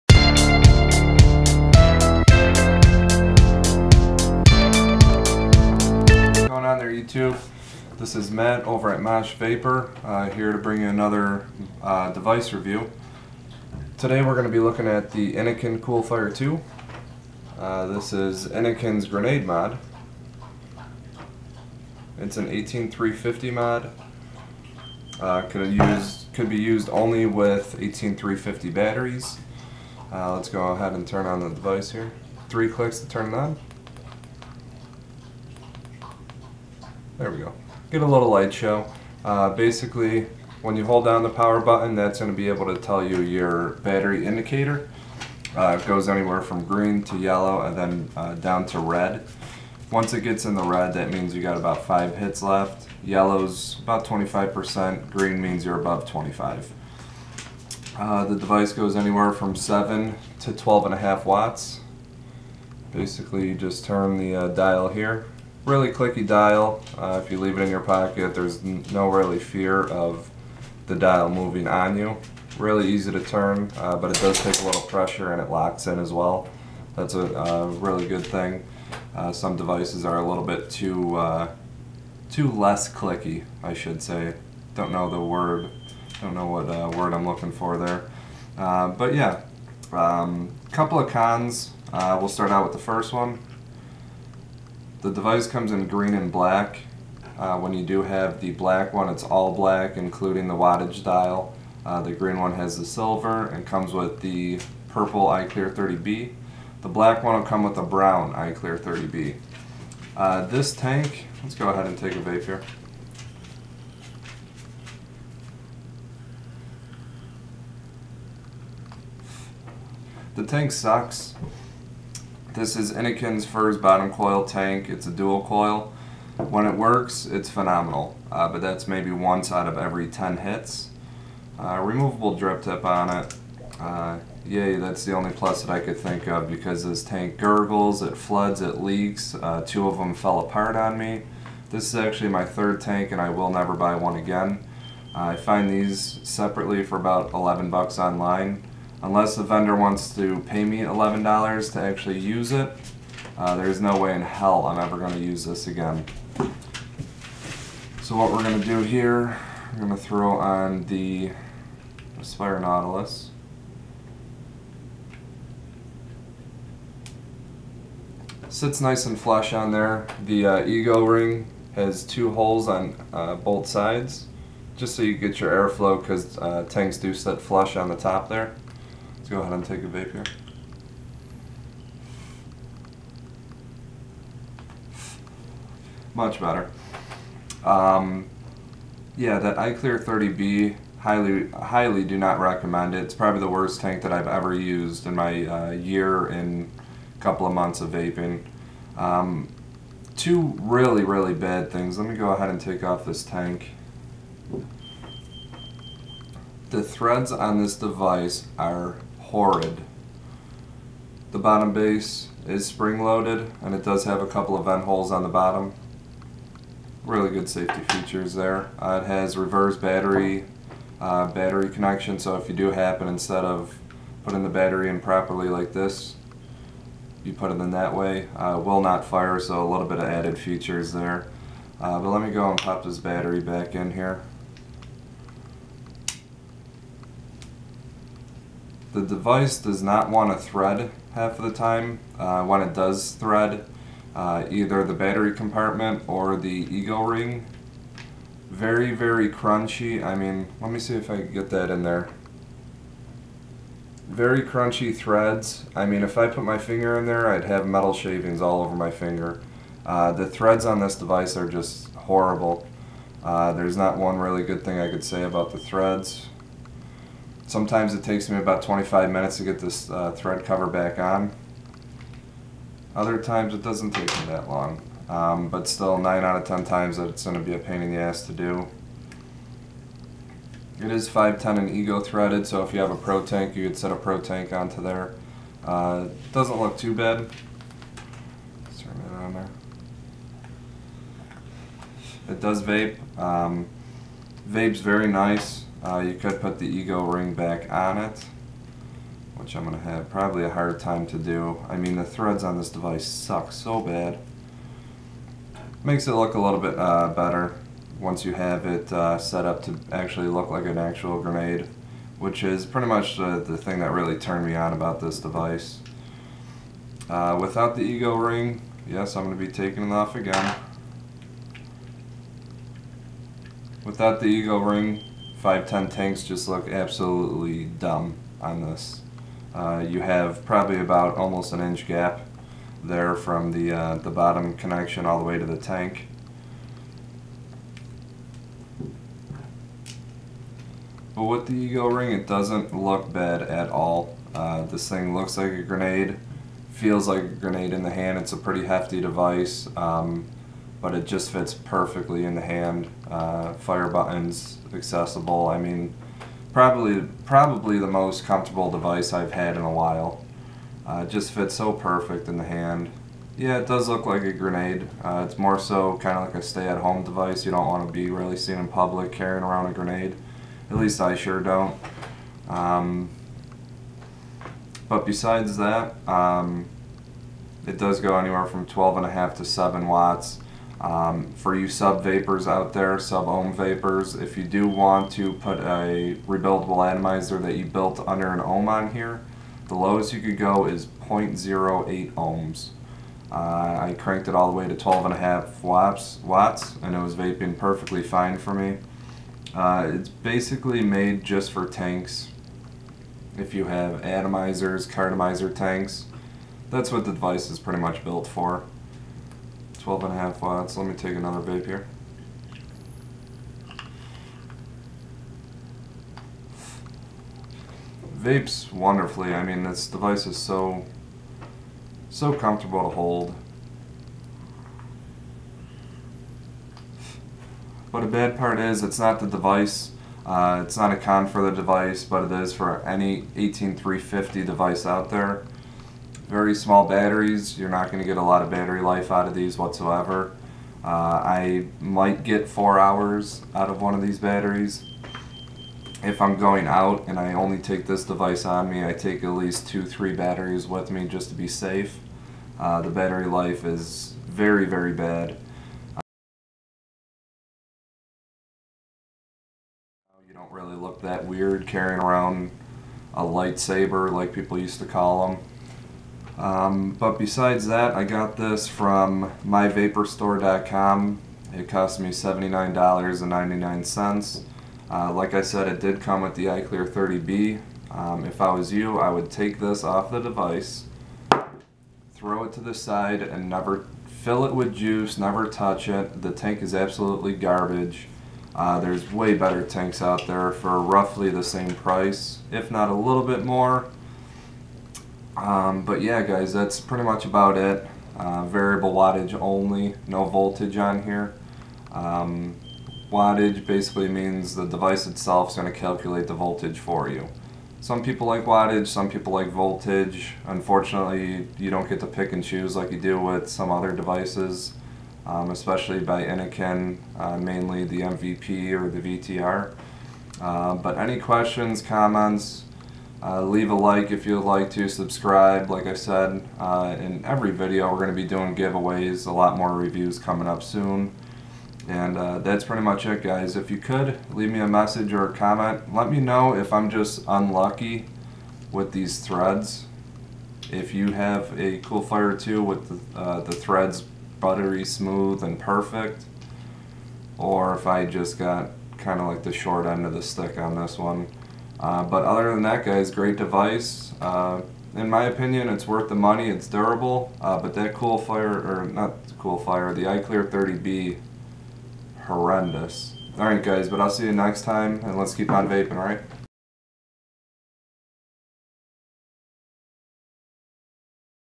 Vaping Device Review!~Innokin Cool Fire 2~
Go to facebook.com and in the search bar, type in moshvapor to see our page there.
Hey guys, Matt here. In this video I show you my pro's and con's about Innokins new device, the Cool Fire 2 that they released late in 2013. Hope you enjoy!
Thanks!!